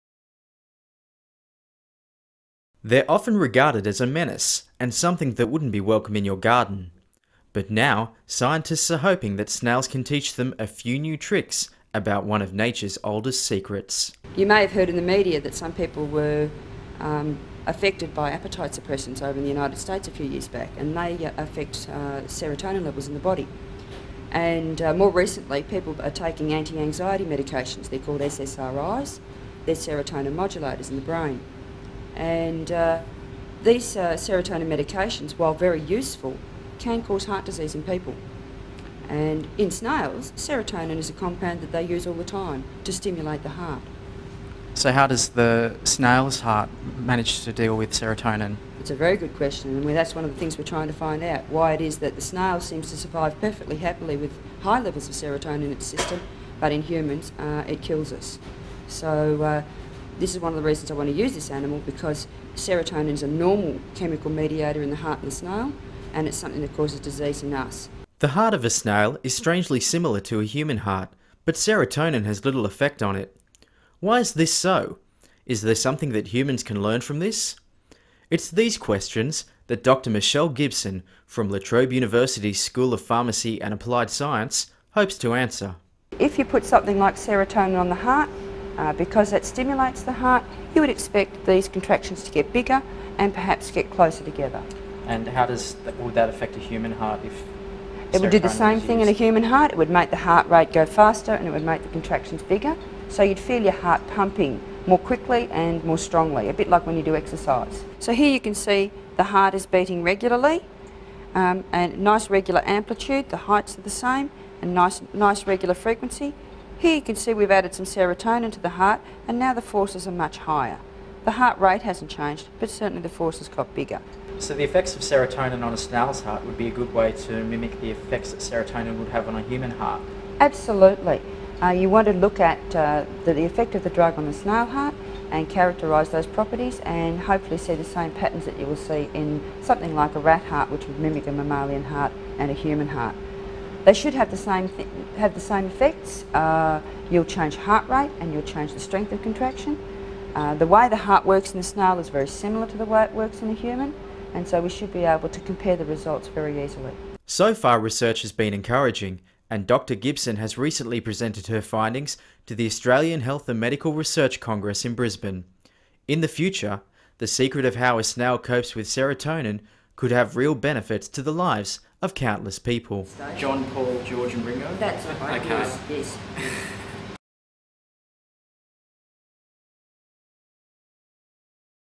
Snails take Serotonin to Heart
Snails could be the key to reducing heart disease says Dr Michelle Gibson from La Trobe University.